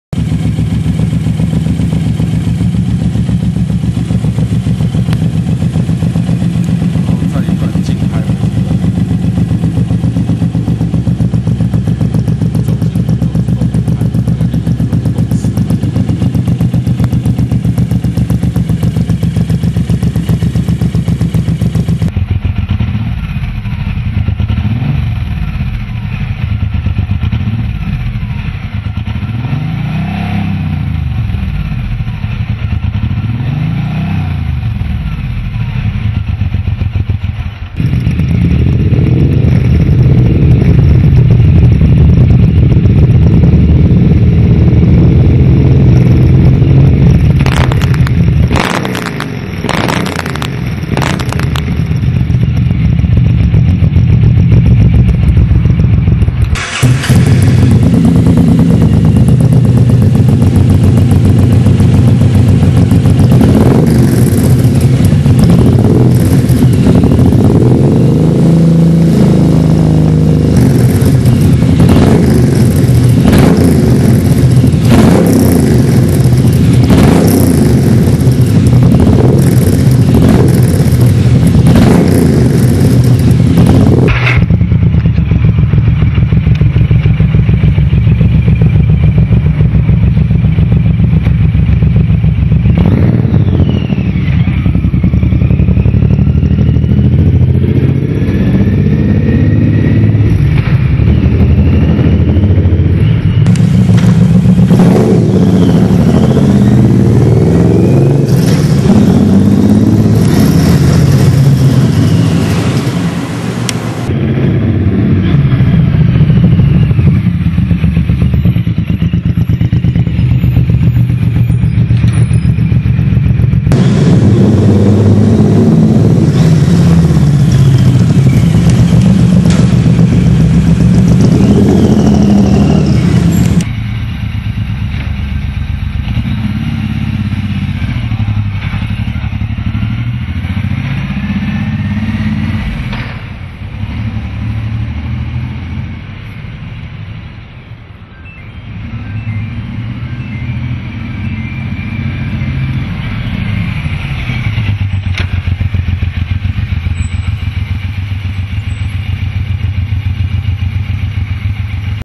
VZR1800 M1800R M109R Exhaust Cannon pipe
This is Cannon 5" 2-1 pipe VS Stock pipe,This pipe design not only for performance ,but also show the subwoofer sound.We use two kinds camera vedio to record the sound.

Revo CANNON

1.Super bass and Subwoofer over all aftermarket muffler. 60Hz realized.
2.Stock exhaust within bracket is weight 66 pound,Cannon pipe only 26.4 pound.
3.Full steel chromed pipe and drum box inner design for subwoofer.
4.Gain 3-5hp,Within air intake and fuel managment would be gain 10-15hp.

Cannon pipes have two models at present. How to purchase?